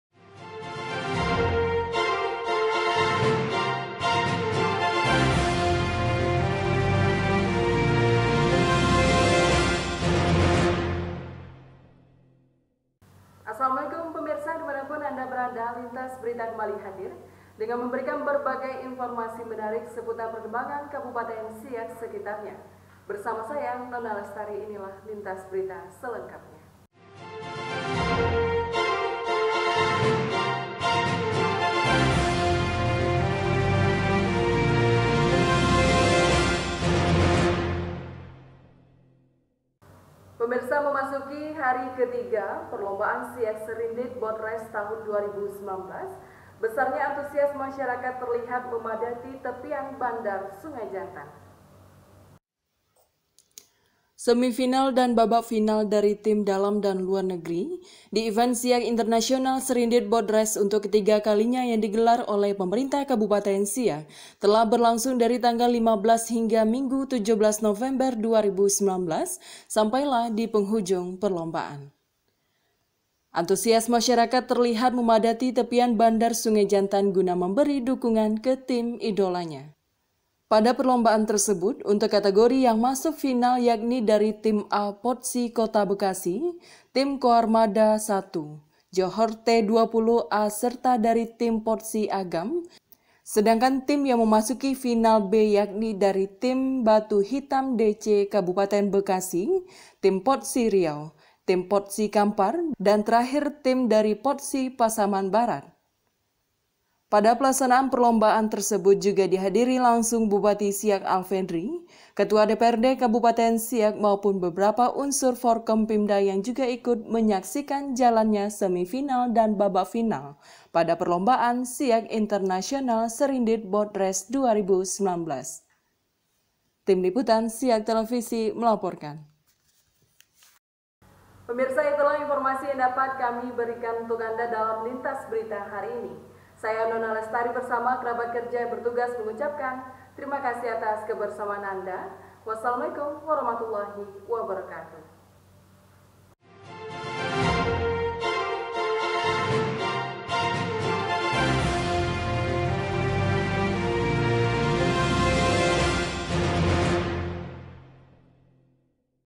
Berita Siak Televisi - Final Siak Internasional Serindit Boat Race
Berita Siak Televisi - Masyarakat Padati Tepian Bandar Sungai Jantan Pada Final Siak Internasional Serindit Boat Race